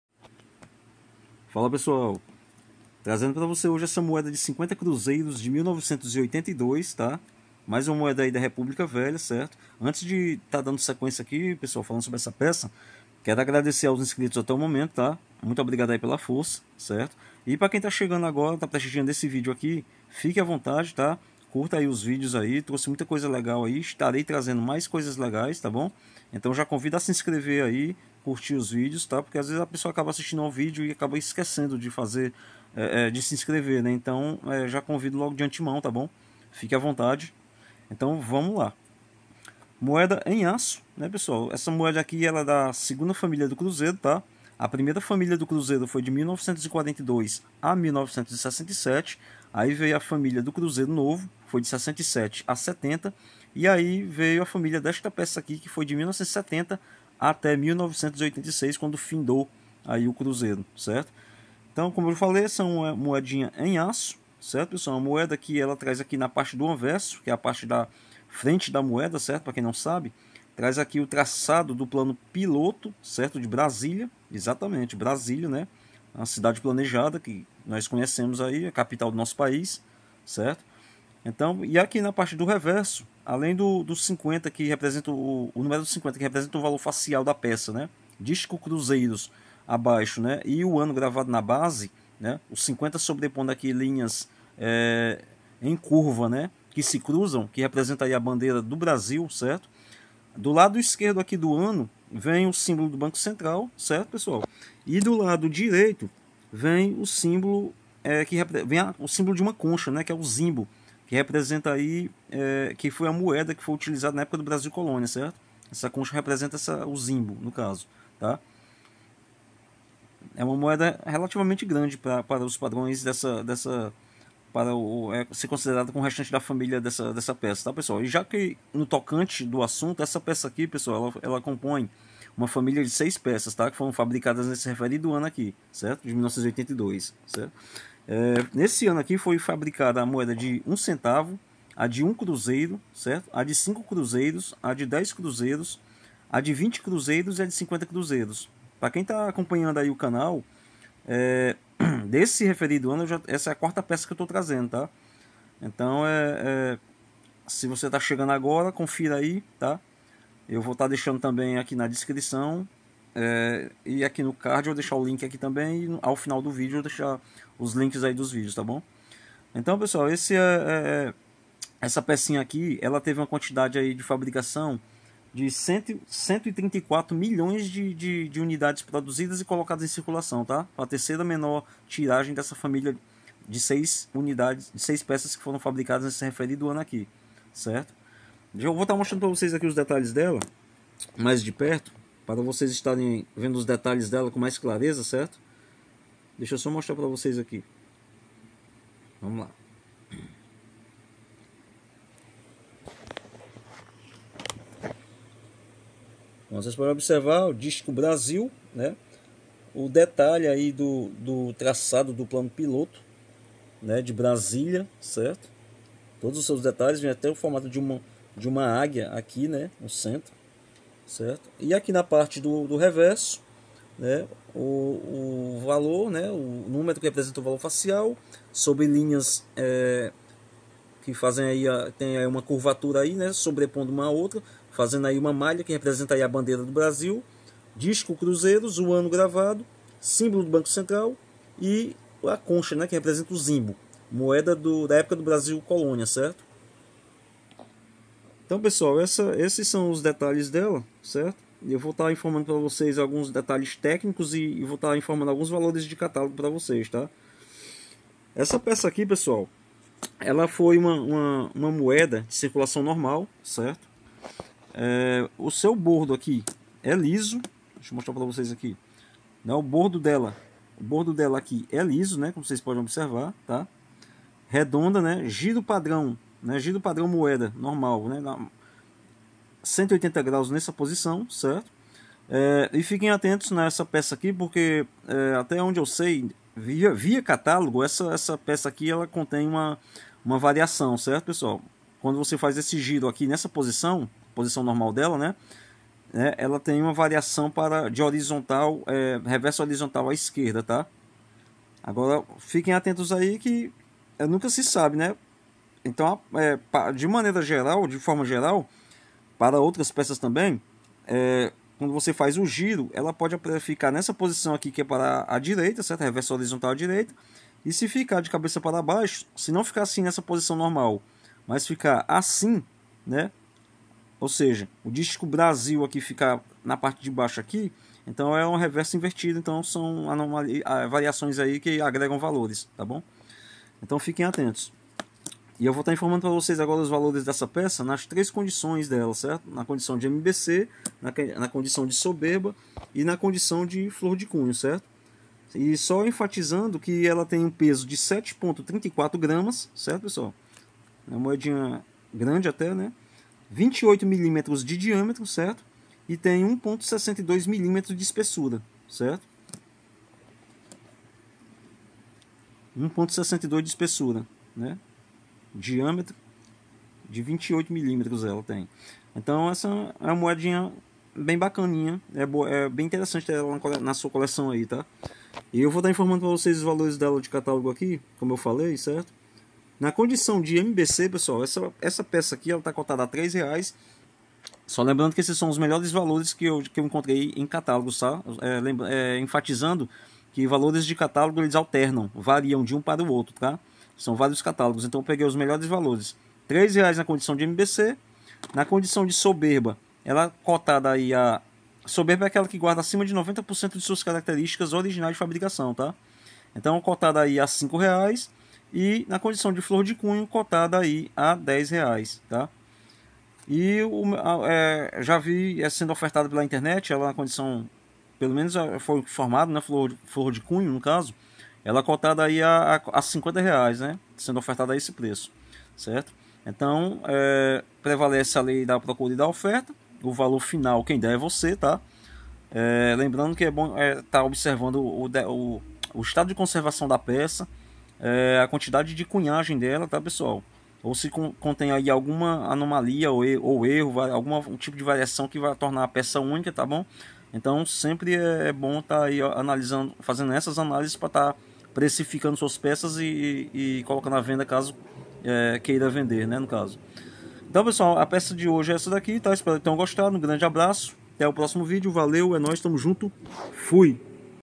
MOEDA DE 50 CRUZEIROS DE 1982 - BRASÍLIA - Valores de Catálogo (Brazilian Coin 50 Cruises of 1982) 🆙
Moeda brasileira de 50 Cruzeiros de 1982, em aço, que faz parte da Segunda Família do Cruzeiro (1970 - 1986) e traz no seu anverso (frente), o traçado do plano piloto de Brasília, a Capital do nosso País.

Para muito mais sobre esta peça, como quantidade de produção, valores e mais, veja o vídeo até o final! 😉👍🏻